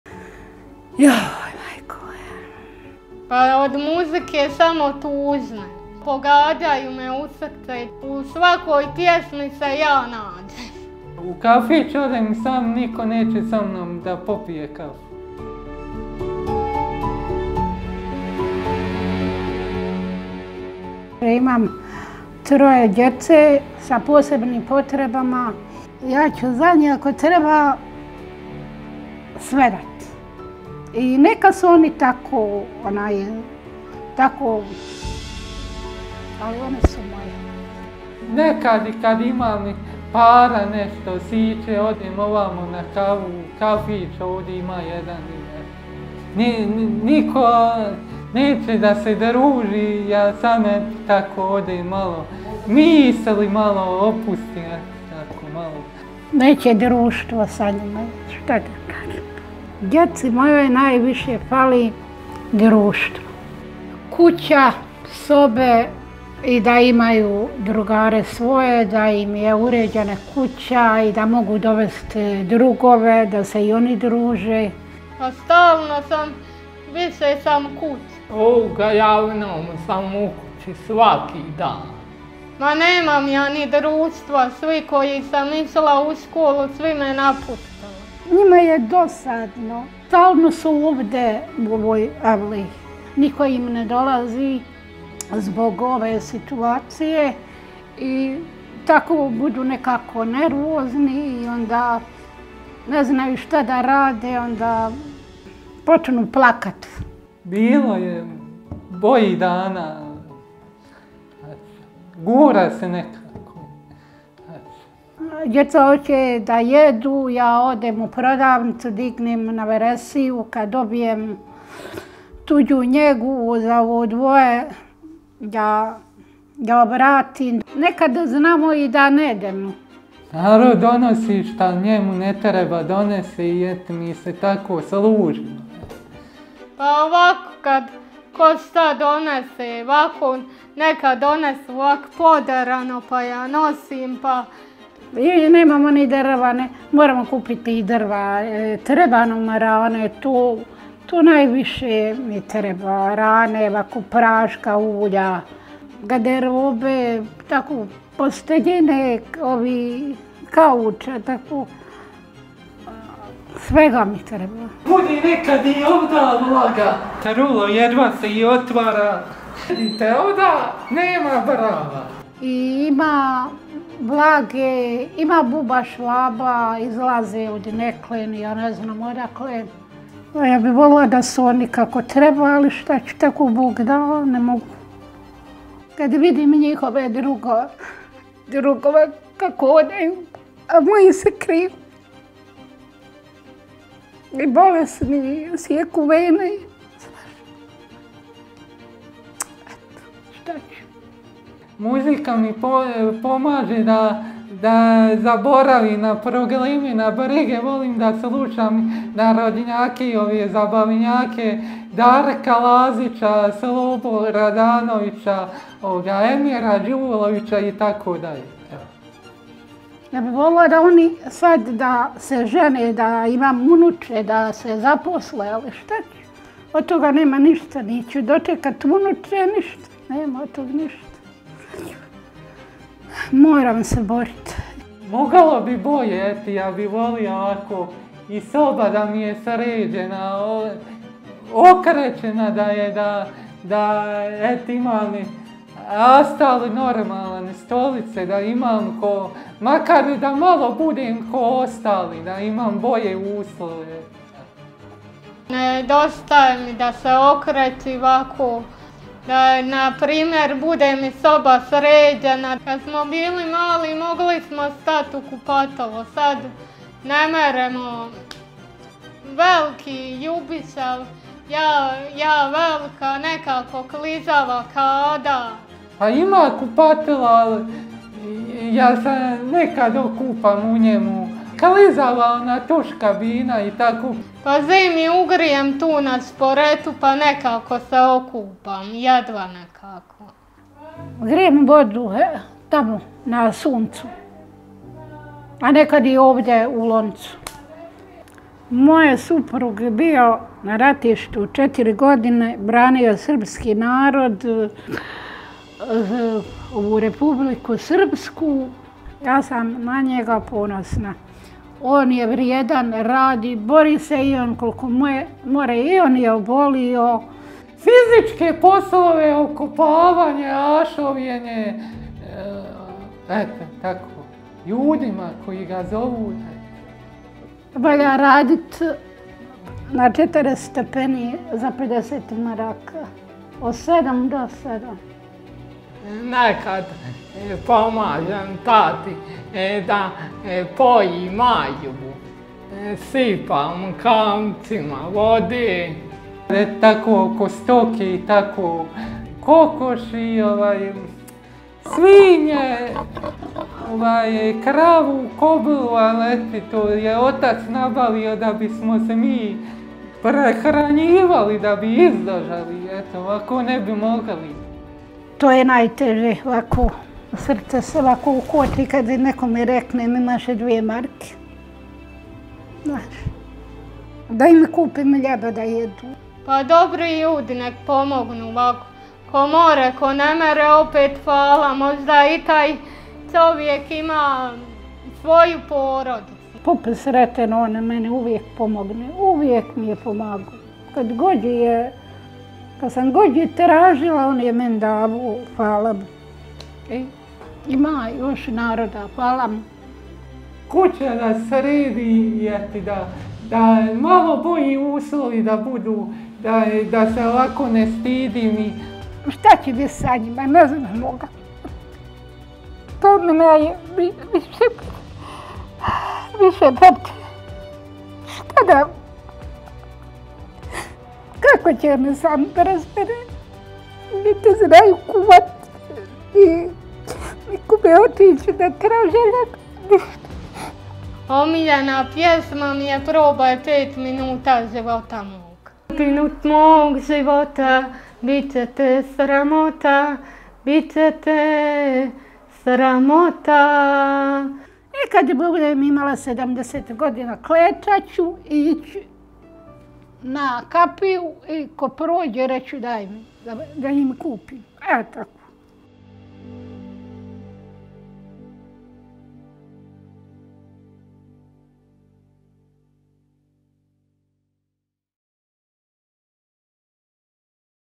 „Јој мајко моја, тежак је живот мој!” – помозимо породици Новаковић!
Болни уздах мајке Зорице која брине о троје дјеце са потешкоћама у развоју. Живану која је рекла да се у свакој тужној пјесми нађе, Љубишу који понекад оде до кафића и пије каву сам и Михајла који ретко када иде из куће, спајају широки осмијеси и снажна воља за животом!

Упознајте Новаковиће и укључите се у акцију ХО Срби за Србе како бисмо им бар обезбиједили боље услове за живот!